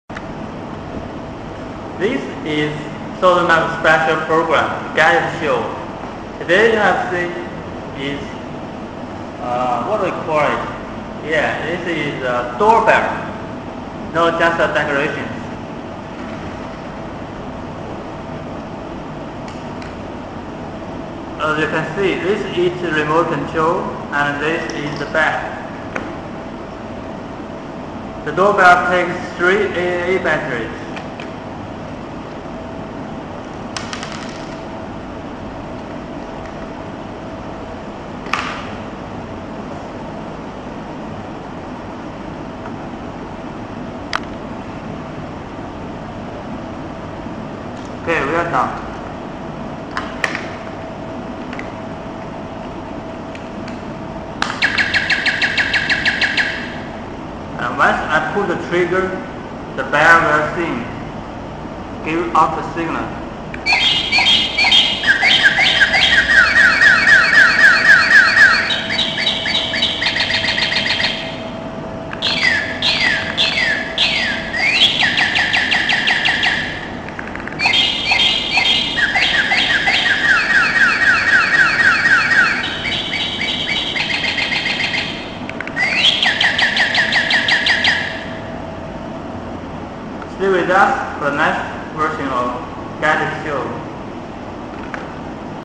Sonerie fara fir in forma de Sticlete Cool-shop.ro
Raza de actiune: 15-20 de metri.

Dimensiunea butonului soneriei: 5.8 x 3 x 1.5 cm

Dimensiunea soneriei Sticlete: 19.5 x 11.5 x 6 cm

Soneria in forma de pasare se alimenteaza cu 3 baterii tip AAAA (nu sunt incluse)

Butonul soneriei se alimenteaza cu o baterie tip AAA (nu este inclusa)

Soneria wireless in forma de sticlete are 12 melodii/triluri de pasare

Butonul soneriei este rezistent la apa

Usor de instalat, pe spatele butonului este o suprafata autocolanta.